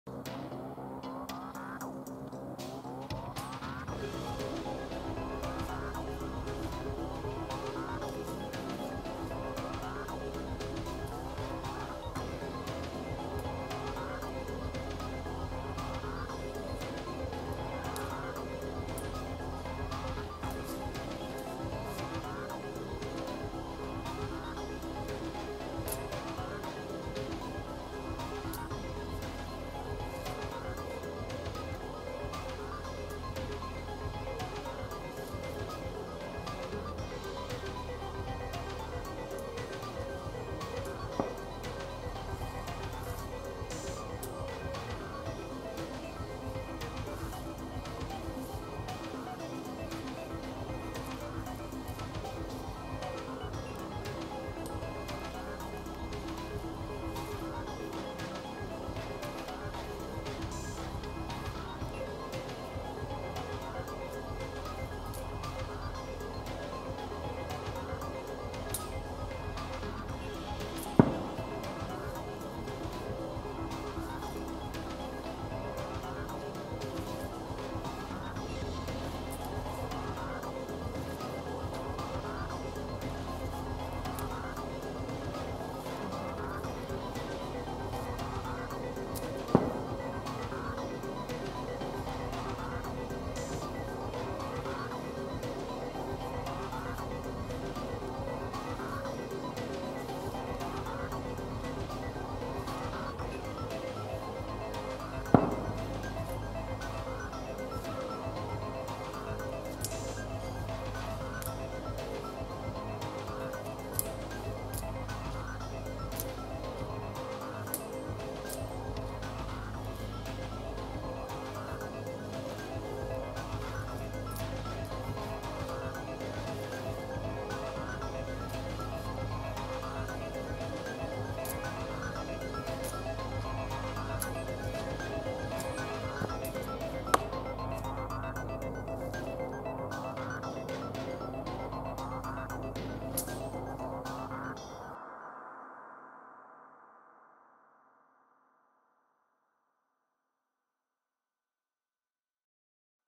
Karabela saber 1.3 kg (interceptions training).
Шабля Карабела 1,3 кг. Втомилася рука робимо переват в іншу.
Володіння историко - традиційними видами зброї,
двобій та самооборона № 46